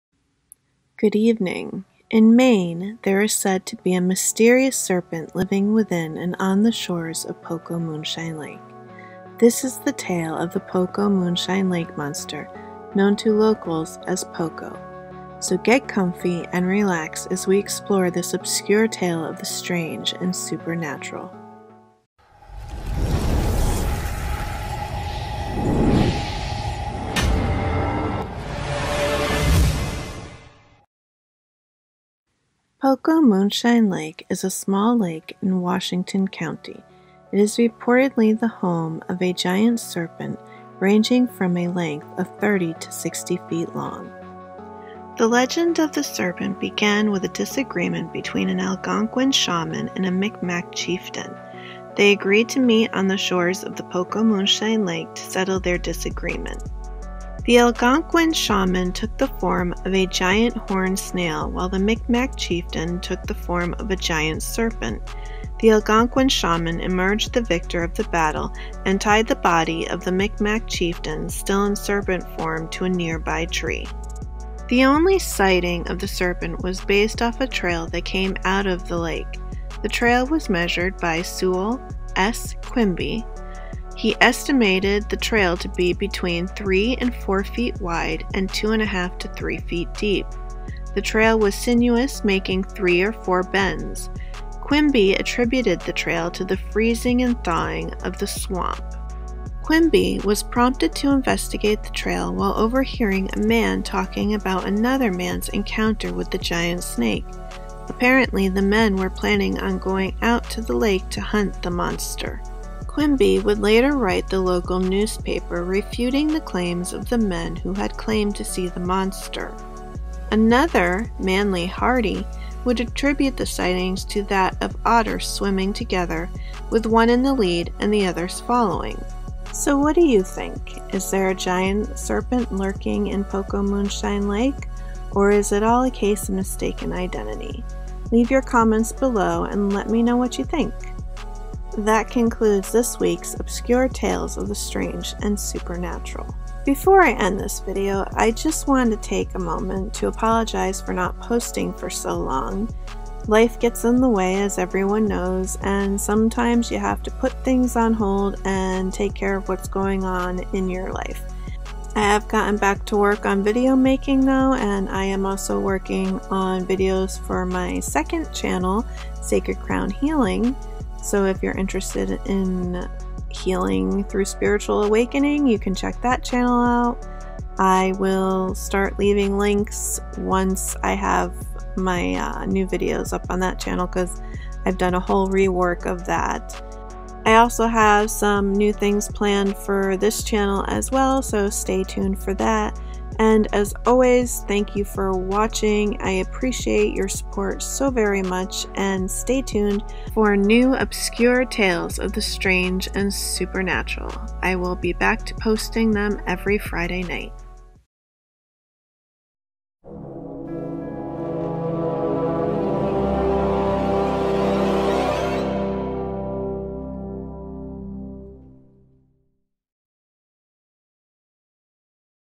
The Pocomoonshine Lake Monster of Maine
Welcome to Obscure Tales of the Strange & Supernatural.
Today I will be sharing the tale of the Pocomoonshine Lake Monster.